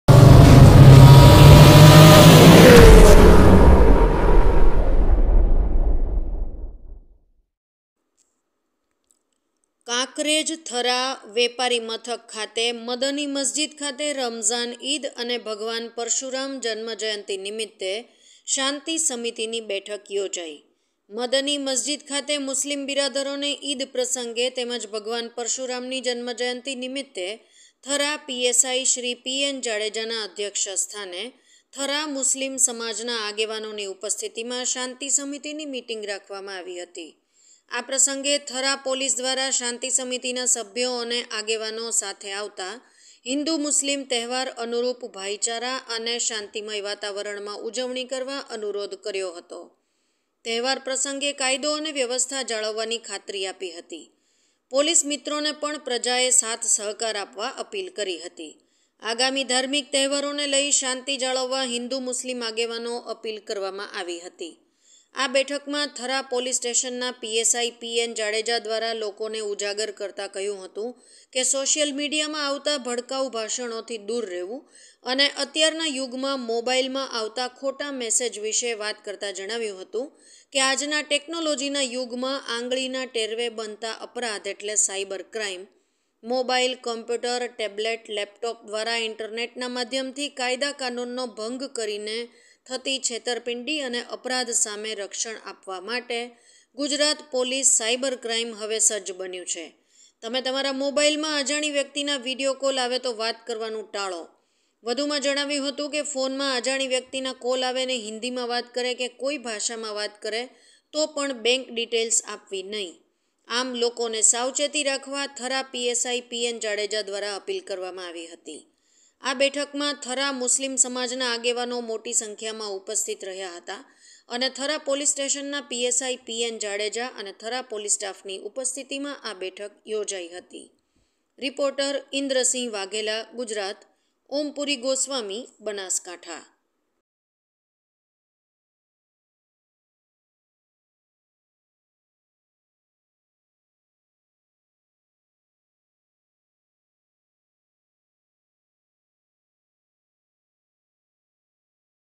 NEWS BANASKANTHA કાંકરેજ થરા મદની મસ્જિદ શાંતી સમિતિ ની બેઠક યોજાઇ
કાંકરેજ થરા  વેપારી મથક ખાતે મદની મસ્જિદ ખાતે  રમઝાન ઈદ અને  ભગવાન પરશુરામ જન્મ જયંતી નિમિત્તે શાંતી સમિતિ ની બેઠક યોજાઇ

મદની મસ્જિદ  ખાતે  મુસ્લિમ બિરાદરોને ઈદ પ્રસંગે તેમજ ભગવાન પરશુરામ ની જન્મ જયંતી  નિમિત્તે થરા પી. એસ આઈ. શ્રી  પી એન .જાડેજા ના અધ્યક્ષ સ્થાને થરા મુસ્લિમ સમાજના  આગેવાનો‌ ની ઉપસ્થિતિ માં શાંતી સમિતિ ની મીટીંગ રાખવામાં હતી, ...
આ પ્રસંગે થરા પોલીસ દ્વારા શાંતિ સમિતિના સભ્યો અને આગેવાનો સાથે આવતા હિન્દુ મુસ્લિમ તહેવાર અનુરૂપ ભાઈચારા અને શાંતિમય વાતાવરણમાં ઉજવણી કરવા અનુરોધ કર્યો હતો. તહેવાર પ્રસંગે કાયદો અને વ્યવસ્થા જાળવવાની ખાતરી આપી હતી અને પોલિસ મિત્રો ને પણ પ્રજાએ સાથ સહકાર આપવા અપીલ કરી હતી. આગામી ધાર્મિક તહેવારો ને લઈ શાંતિ જાળવવા હિન્દુ મુસ્લિમ આગેવાનો અપીલ કરવામાં આવી. હતી...

આ બેઠક માં થરા પોલીસ સ્ટે ના  PSI પી.એન.જાડેજા દ્રારા લોકો ને  ઉજાગર કરતા કહ્યું હતું કે સોશિયલ મીડિયા માં આવતા ભડકાઉ ભાષણો થી દૂર રહેવું અને અત્યાર ના ઉગ મા મોબાઈલ માં આવતા ખોટા  મેસેજ  વિશે વાત કરતા  જણાવ્યું   હતું કે આજના ટેક્નોલોજીના યુગમાં આંગળીના ટેરવે બનતા અપરાધ એટલે સાયબર ક્રાઇમ. મોબાઇલ, કોમ્પ્યુટર, ટેબલેટ, લેપટોપ દ્વારા ઇન્ટરનેટના માધ્યમથી કાયદા-કાનુનનો ભંગ કરીને થતી છેતરપીંડી અને અપરાધ સામે રક્ષણ આપવા માટે ગુજરાત પોલીસ સાયબર ક્રાઇમ હવે સજ્જ બન્યું છે. તમે તમારા મોબાઈલ માં અજાણી વ્યક્તિ ના વિડિયો  કોલ આવે તો  વાત કરવાનું ટાળો  ત્યારે વધુ જણાવ્યું હતું કે  તમારા ફોન માં અજાણી વ્યક્તિ ના કોલ આવે અને હિન્દી માં વાત કરે  કે કોઈ ભાષા  માં વાત કરે તો પણ બેંક ડિટેઇલ્સ આપવી નહીં. આમ લોકો ને સાચવતી રાખવા થરા PSI પી. એન જાડેજા દ્રારા અપીલ કરવામાં આવી હતી...

આ બેઠક માં થરા મુસ્લિમ સમાજ ના આગેવાનો મોટી સંખ્યામાં ઉપસ્થિત રહ્યા હતા અને થરા પોલીસ સ્ટેશન ના PSI પી. એન જાડેજા અને થરા પોલીસ સ્ટાફ ની ઉપસ્થિત માં આ બેઠક યોજાઈ હતી..

રિપોર્ટર...
ઈન્દ્રસિંહ વાઘેલા.. ગુજરાત
ઓમપુરી ગોસ્વામી.. બનાસકાંઠા